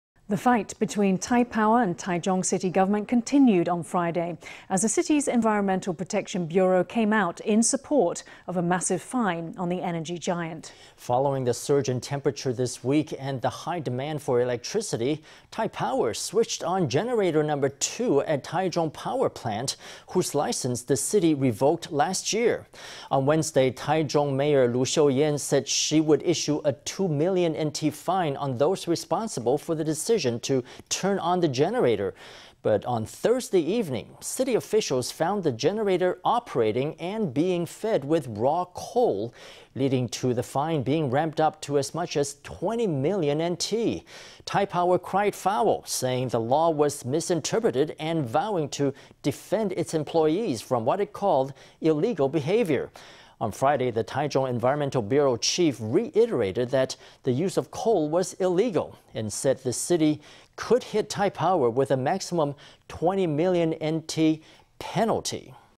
Taichung ramps up fine on Taipower after Taichung Power Plant generator is restarted
The fight between Taipower and Taichung City government continued on Friday, as the city''s Environmental Protection Bureau came out in support of a massive fine on the energy giant. Following the surge in temperature this week and the high demand for electricity, Taipower switched on generator no. 2 at Taichung Power Plant, whose license the city revoked last year.

On Wednesday, Taichung Mayor Lu Shiow-yen  said she would issue a NT$2 million fine on those responsible for the decision to turn on the generator. But on Thursday evening, city officials found the generator operating and being fed with raw coal, leading to the fine being ramped up to as much as NT20 million. Taipower cried foul, saying the law was misinterpreted and vowing to defend its employees from what it called "illegal behaviour". On Friday the Taichung Environmental Bureau chief reiterated that the use of coal was illegal, and said the city could hit Taipower with a maximum NT$20 million penalty.